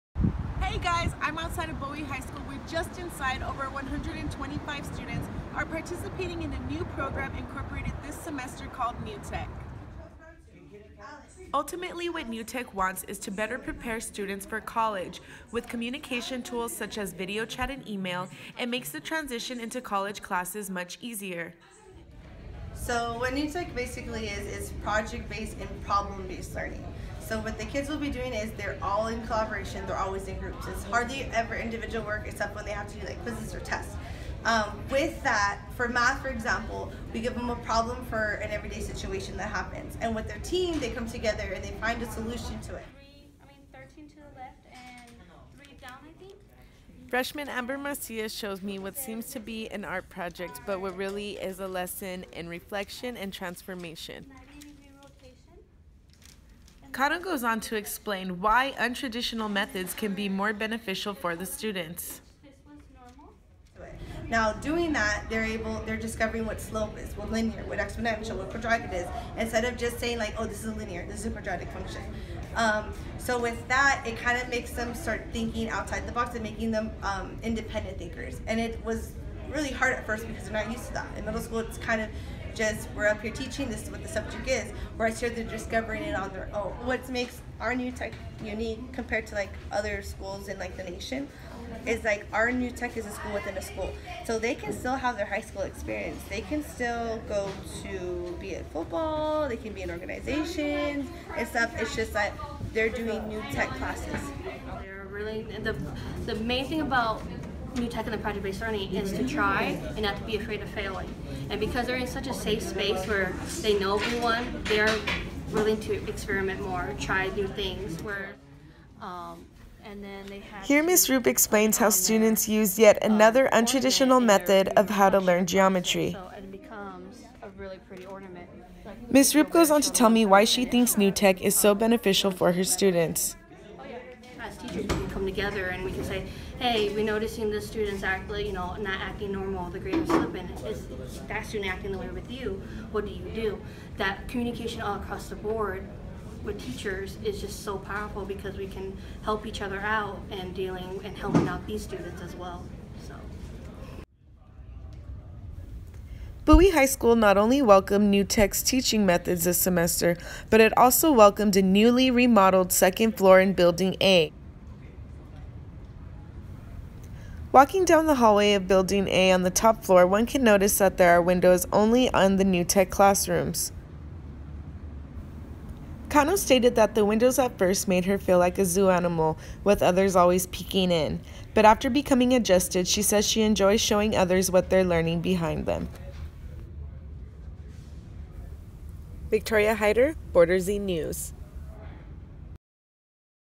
A look inside the New Tech program at Bowie High School
Borderzine reporter Victoria Hyder goes inside the New Tech program at Bowie High to see how it is preparing students for the transition to college.